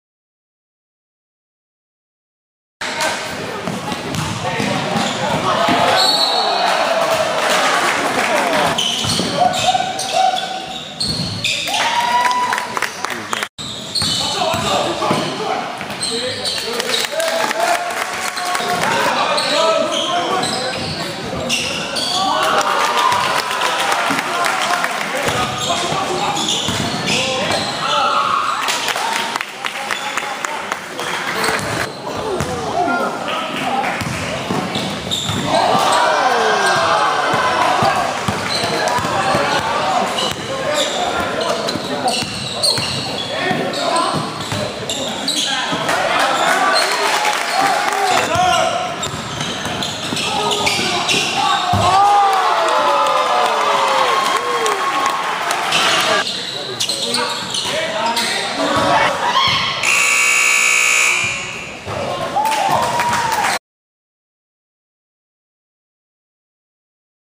MiKyle McIntosh - 22 Feet Academy - Dominates with UCLA Coach in Stands
2013 star MiKyle McIntosh of 22 Feet Academy (KY) dominates Wesley Christian with UCLA assistant Korey McCray in attendance.